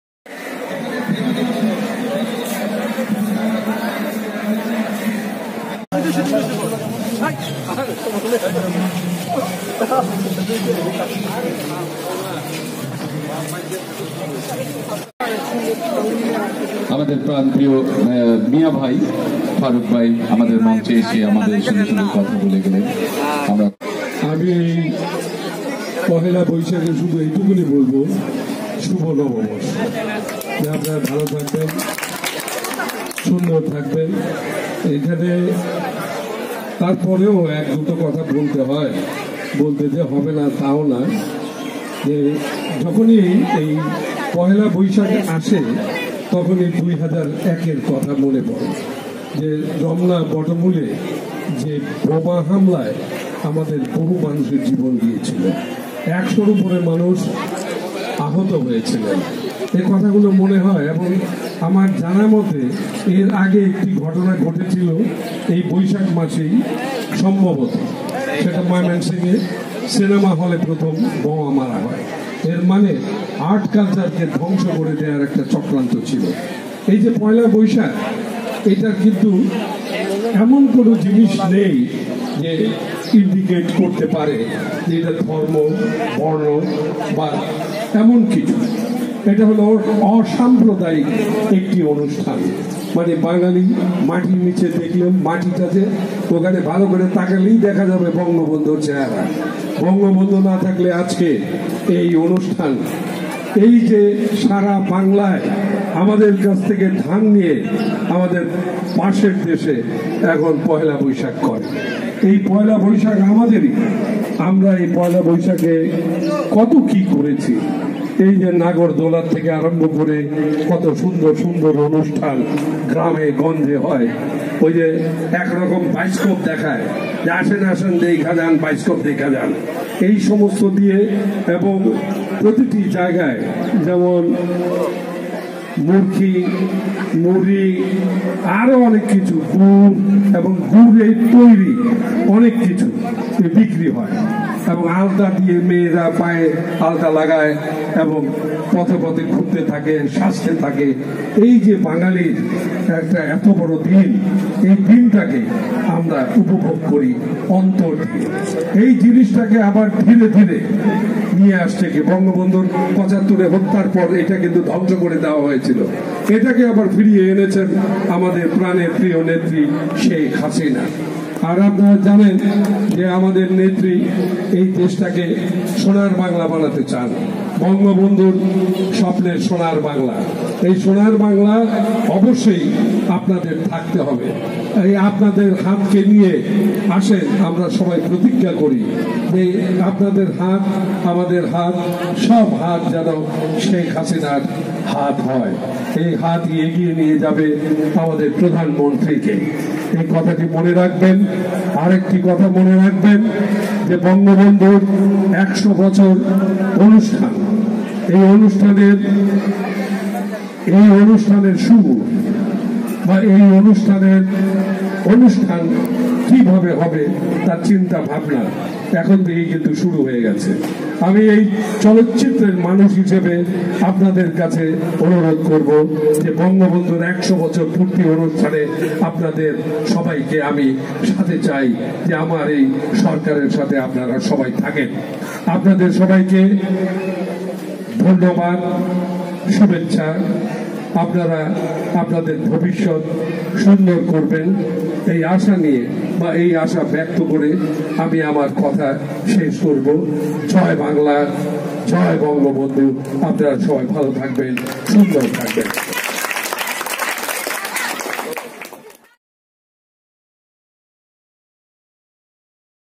চলচ্চিত্র পরিবারে বৈশাখী উৎসব ১৪২৬
পহেলা বৈশাখের দিন সারাদিন বাংলাদেশ চলচ্চিত্র উন্নয়ন কর্পোরেশন (বিএফডিসি) তে বৈশাখী উৎসব ও উৎসবে সংসদ সদস্য চিত্রনায়ক ফারুক এর বক্তব্য নিয়ে আমাদের এই ভিডিও।